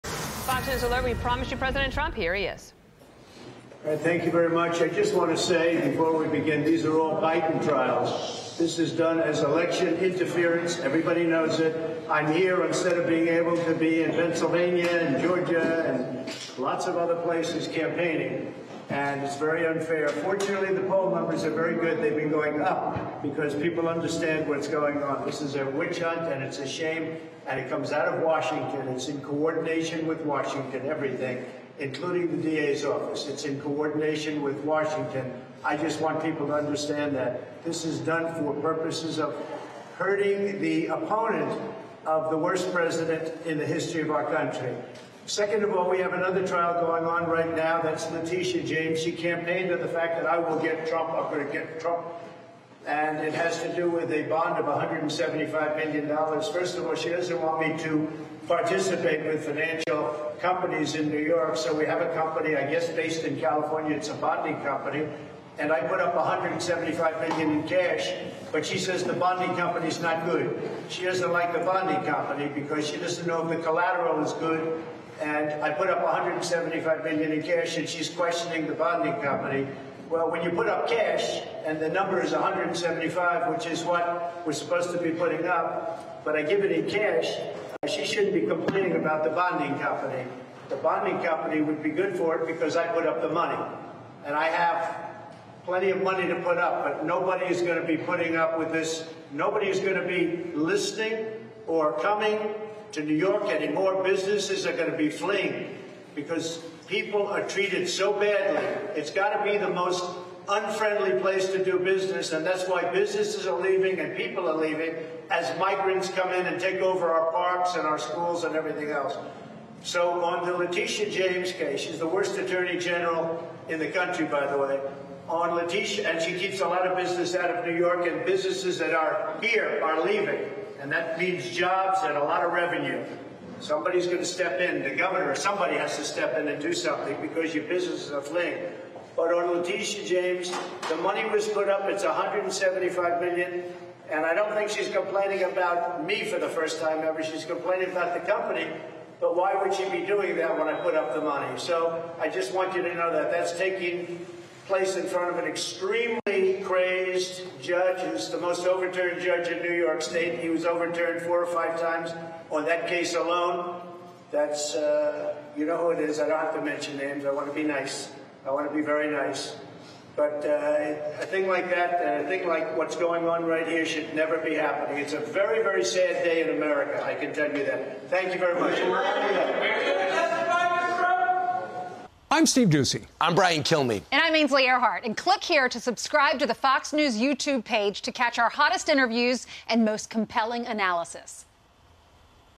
Trump blasts 'witch hunt' hush money trial: 'This is in coordination with Washington'
Former President Donald Trump delivers remarks to press ahead of opening statements in the hush money trial in New York City.